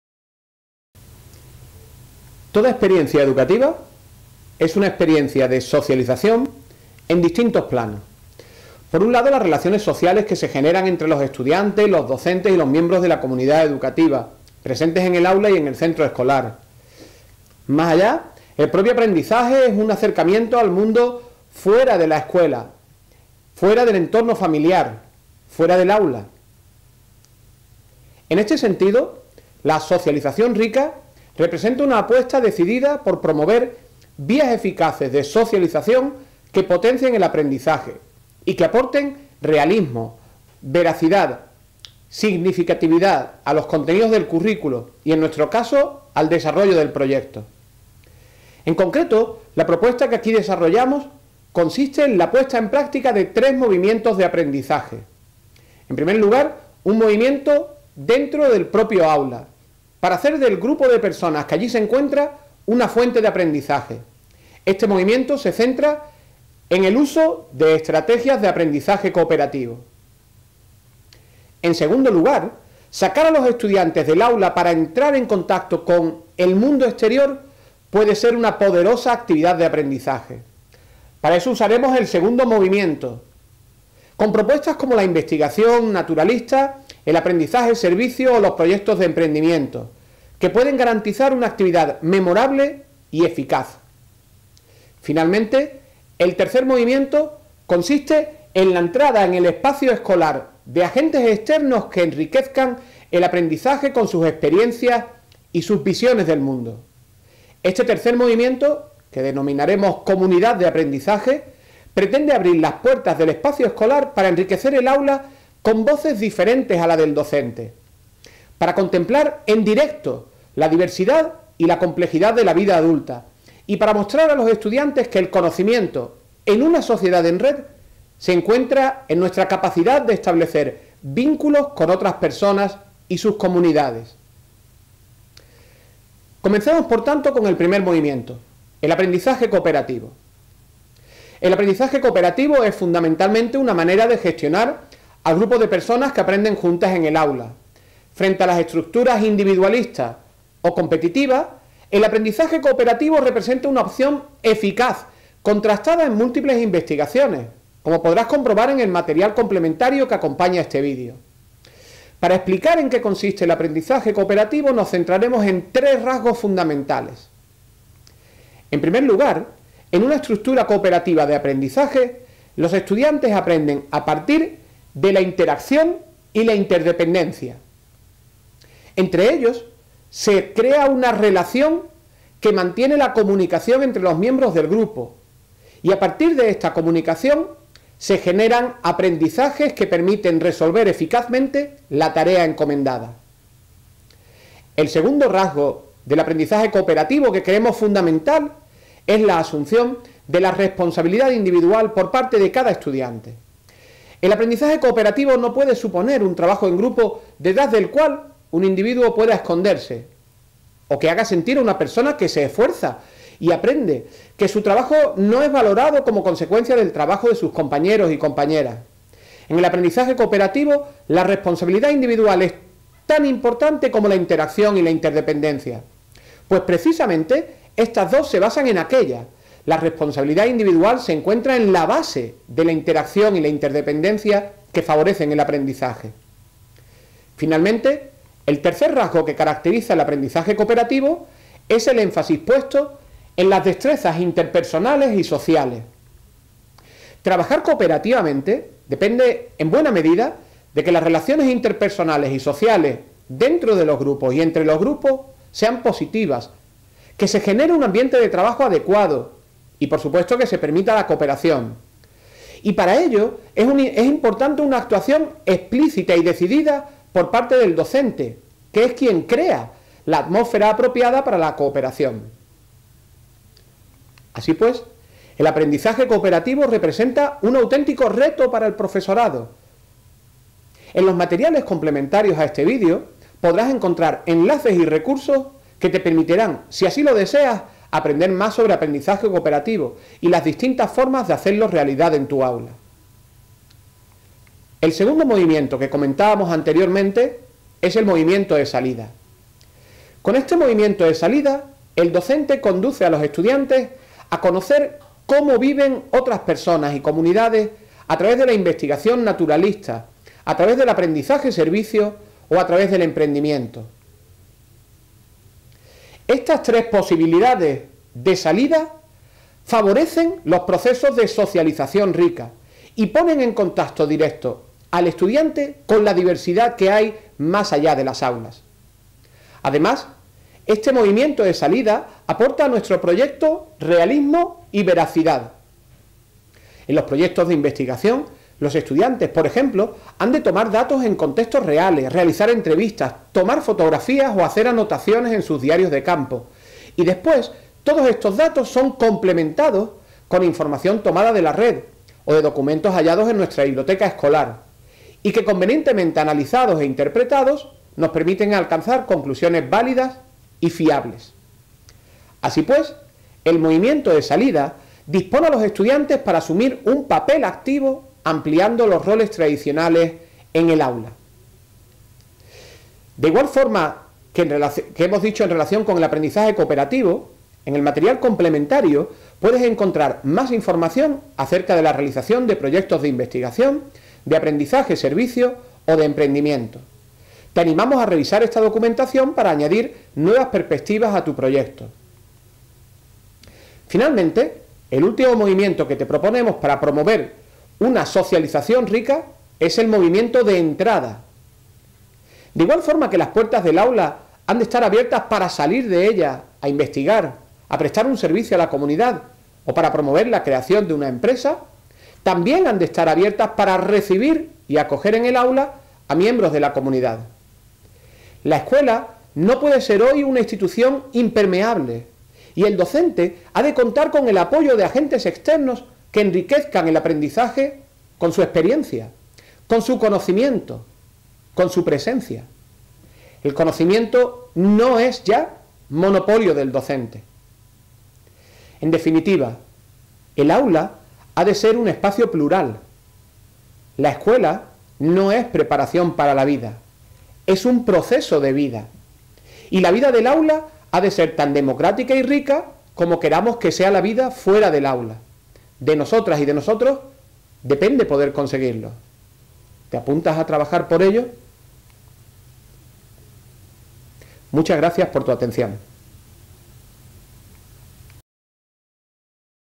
Vídeo 3.2. "Abriendo el proyecto (I): la socialización rica" - Ideas Clave
Vídeo de ideas clave de la unidad 3 del MOOC de Aprendizaje Basado en Proyectos ABPmooc_intef.